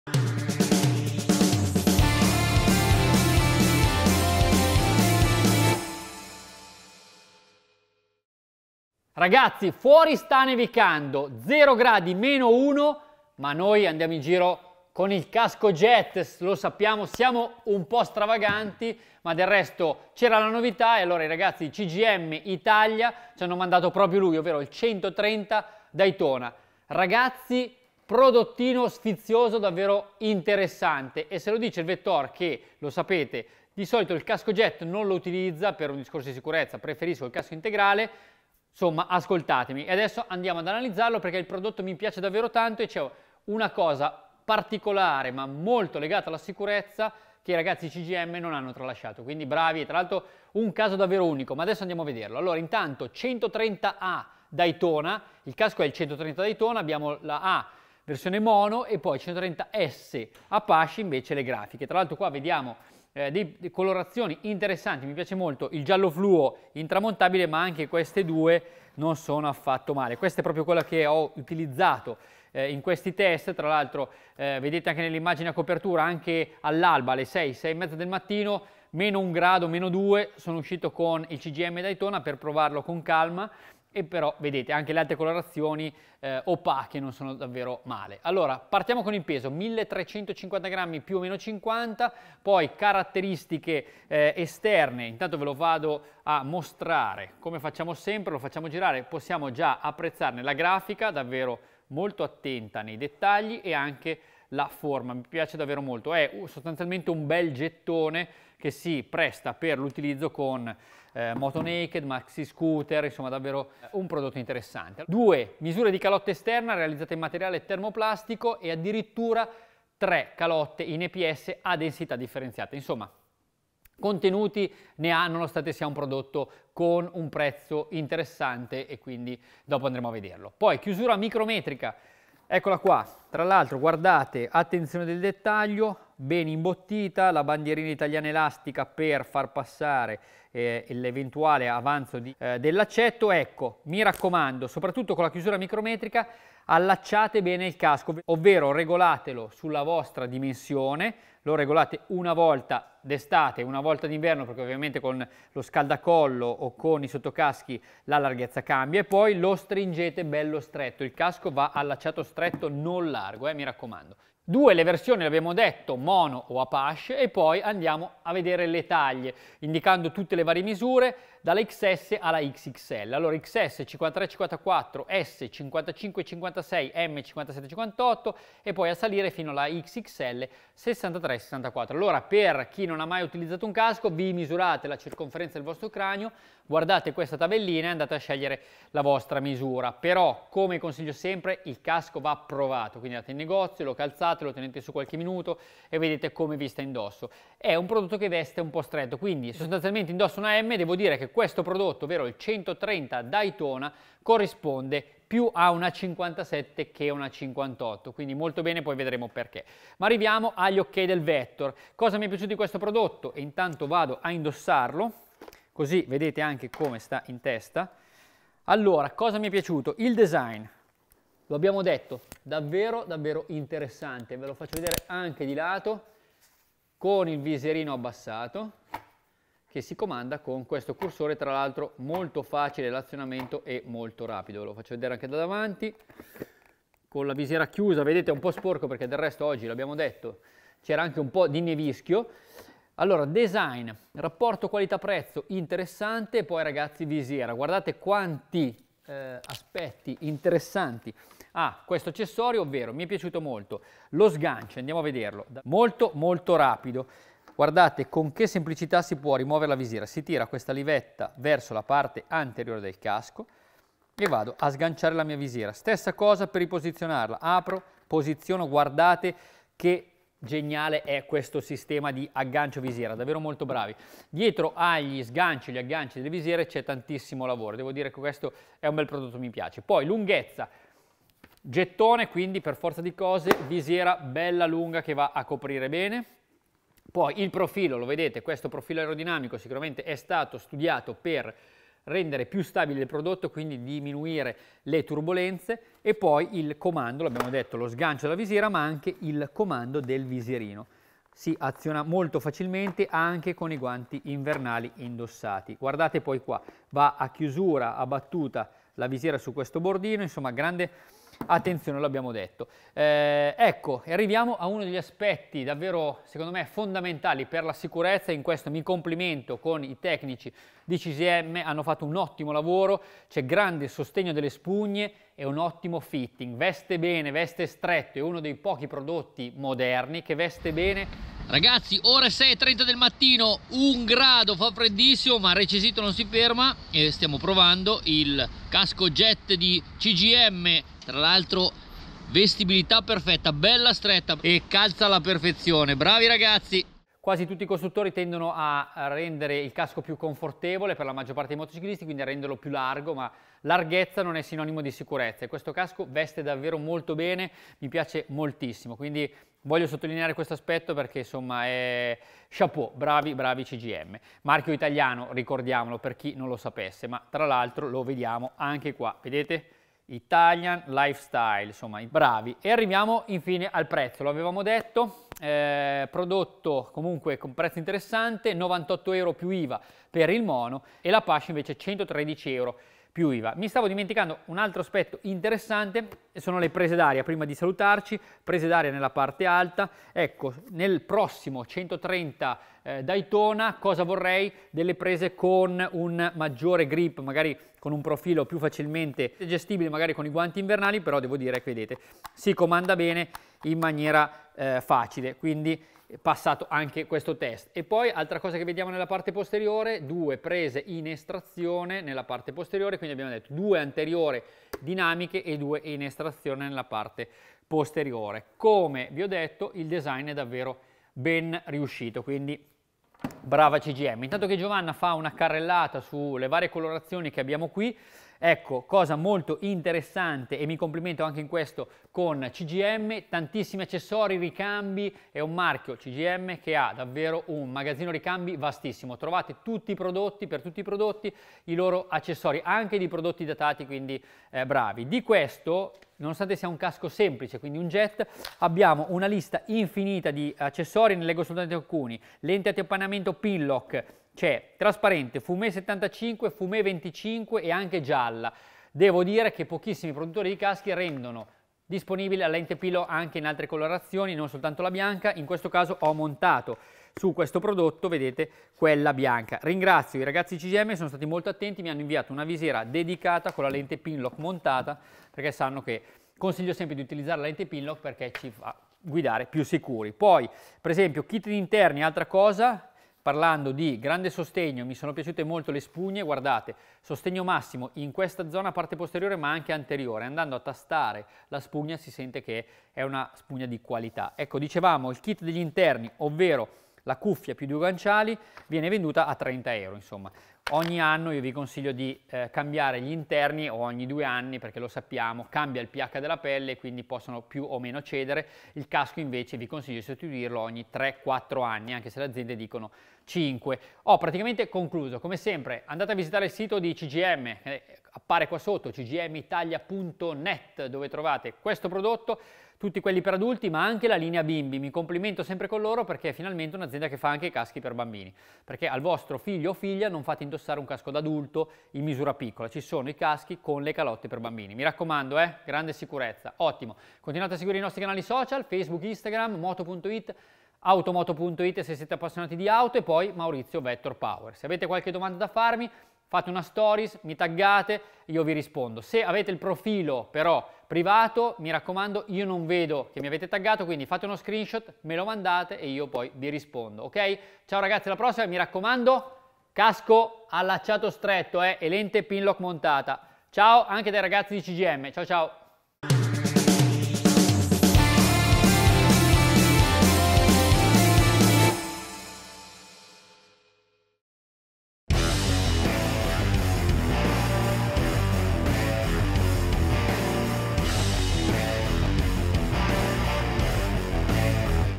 CGM 130 Daytona. Recensione casco jet urban-touring da 98 euro!
Ecco la nuova proposta di CGM Italia. Si chiama 130 Daytona ed è il nuovo "jettone" con calotta realizzata in due misure in materiale termoplastico. L'ho provato e analizzato per voi, nonostante le temperature rigide. Il casco mi è piaciuto molto per la linea ma soprattutto per gli interni che sanno dare tanto confort con un'ottima distribuzione della pressione sul cranio. Senza contare che veste piuttosto stretto, un vantaggio in termini di sicurezza! Brava CGM. Prezzi a partire da 98 euro + iva.  Info